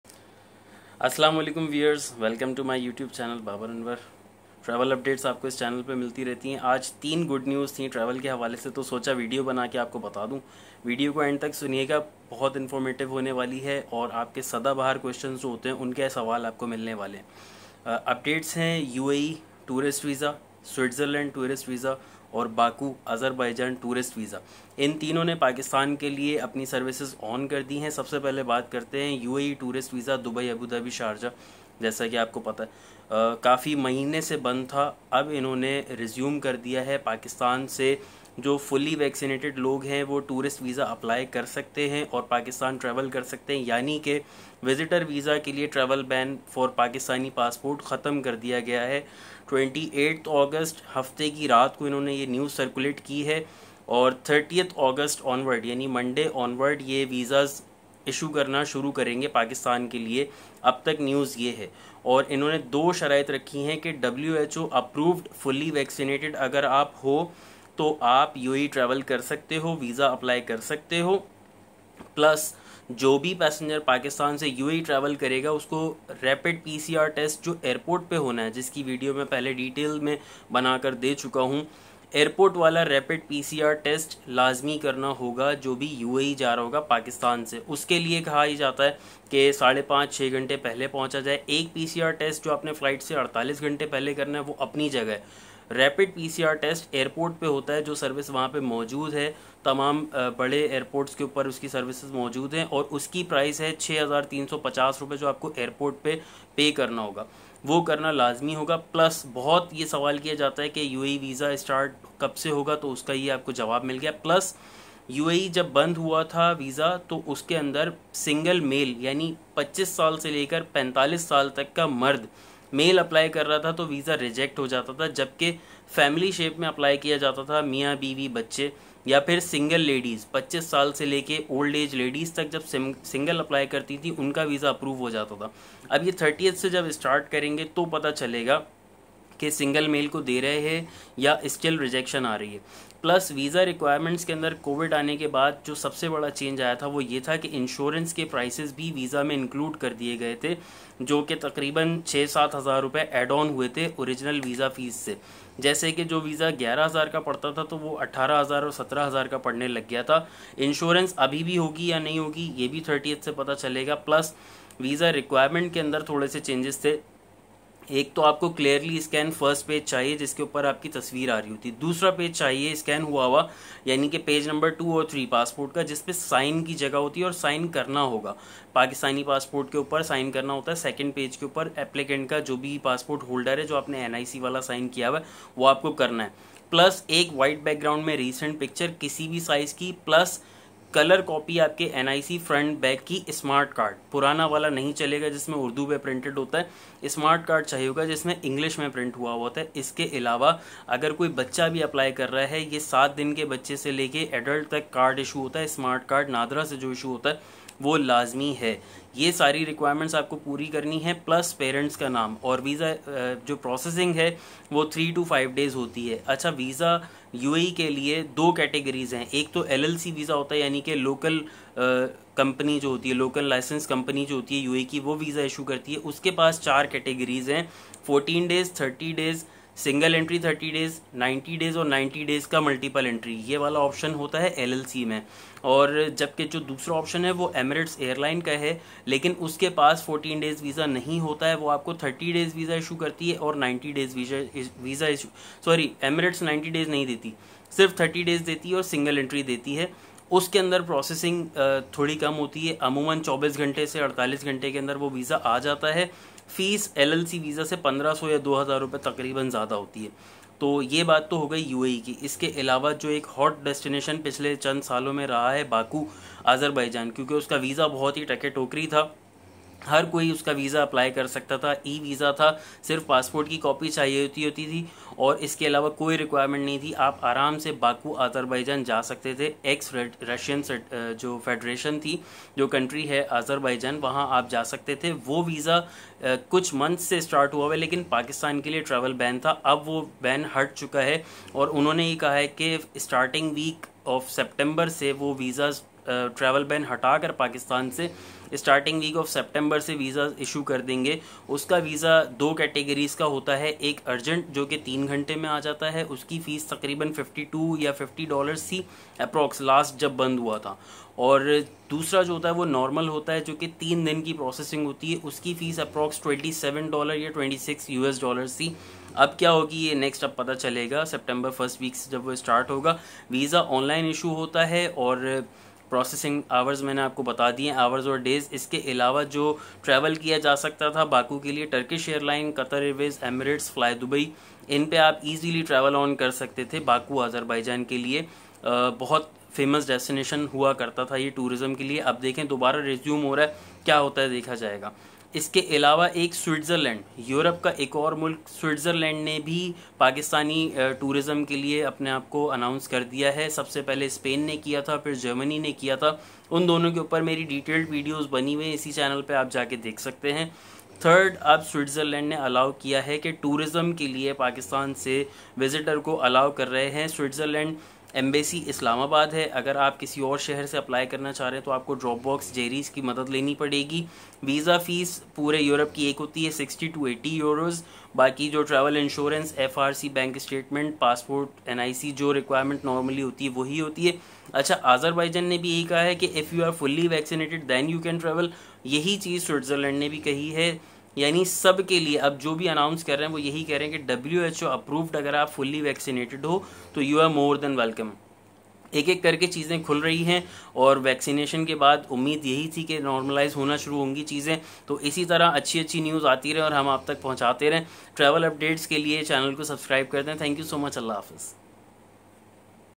Dubai Visit Visa Open, UAE Tourist Visa | Baku, Azberbaijan | Switzerland Europe | Tourism | Travel
I am informing about the recent announcement from Dubai, UAE, visitor tourist visa, Baku, Azerbaijan must awaited tourist destination and Switzerland, Europe Schengen State tourism open for Pakistan